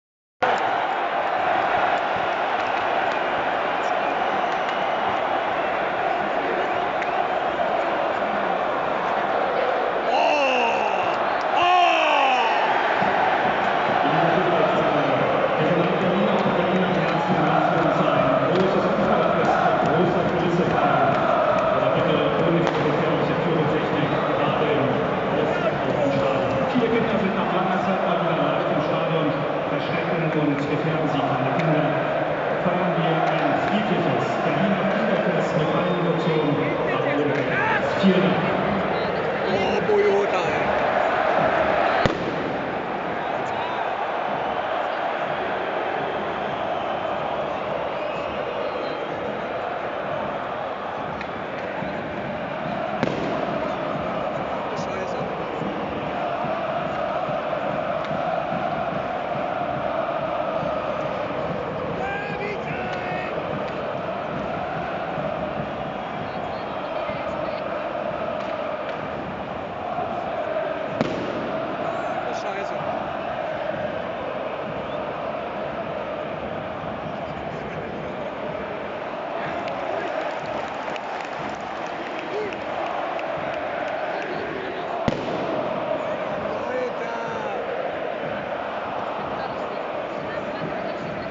Rückkehr der Hertha BSC Ultras in die Ostkurve gegen den 1. FC Union
Am 09. April 2022 war es endlich wieder soweit und die Hertha BSC Ultras kehrten mit Pyro, Rauchwolken und einer Choreo in die Ostkurve des Berliner Olympiastadions zurück brim Derby Hertha BSC gegen Union Berlin